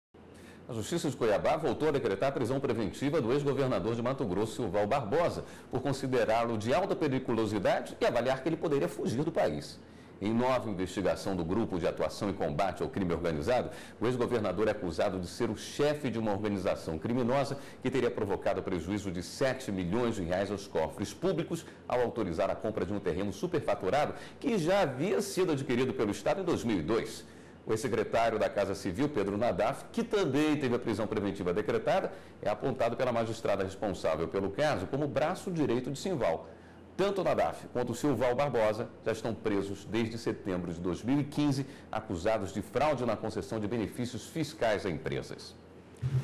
Nota governador MT (02/02/16)
A Justiça de Cuiabá voltou a decretar a prisão preventiva do ex-governador de Mato Grosso, Silval Barbosa, por considerá-lo de alta periculosidade e avaliar que ele poderia fugir do país. Em nova investigação do grupo de atuação e combate ao crime organizado, o ex-governador é acusado de ser o chefe de uma organização criminosa que teria provocado prejuízo de 7 milhões de reais aos cofres públicos ao autorizar a compra de um terreno superfaturado que já havia sido adquirido pelo estado em 2002. O ex-secretário da casa civil, Pedro Nadaf, que também teve a prisão preventiva decretada, é apontado pela magistrada responsável pelo caso como braço direito de Silval. Tanto Nadaf quanto Silval Barbosa já estão presos desde setembro de 2015, acusados de fraude na concessão de benefícios fiscais a empresas.